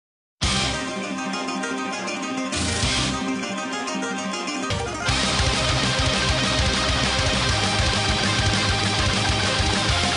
Banjo skeletons
Skeletons playing dragon force